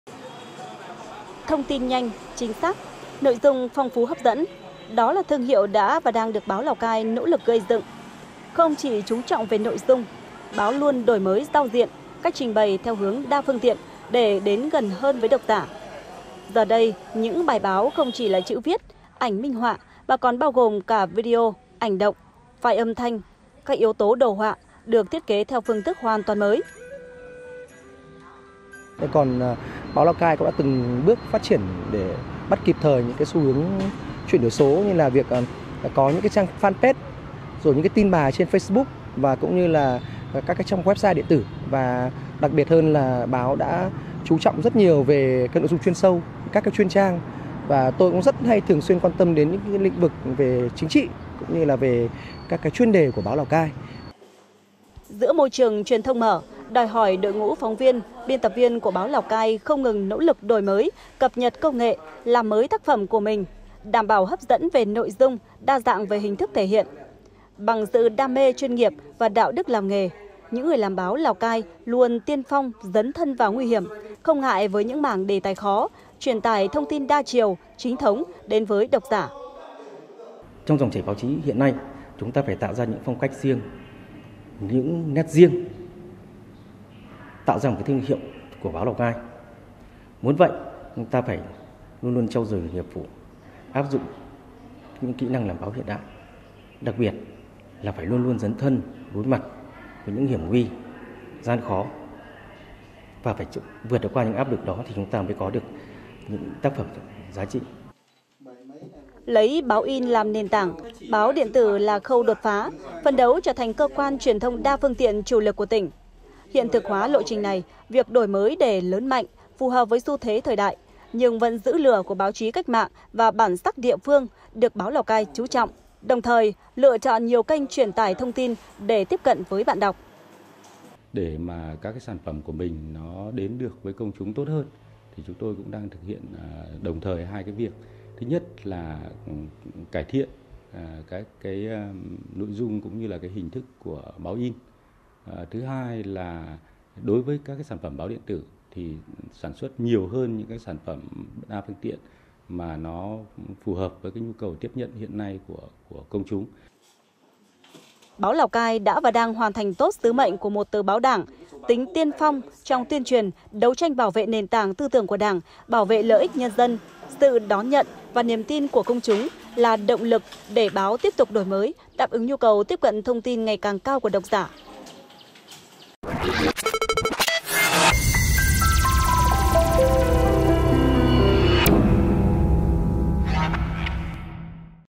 Báo Lào Cai đổi mới để đến gần hơn với công chúng | THLC
Laocaitv.vn - Cuộc cách mạng công nghiệp 4.0, với việc bùng nổ mạnh mẽ của mạng xã hội đặt ra không ít thách thức đối với báo chí truyền thống, kể cả các trang báo điện tử. Đứng trước áp lực "phải thay đổi" để khẳng định mình, Báo Lào Cai chủ động đón nhận thách thức và cơ hội của thời đại công nghệ số, quyết tâm trở thành cơ quan truyền thông đa phương tiện.
Đài Phát thanh - Truyền hình Lào Cai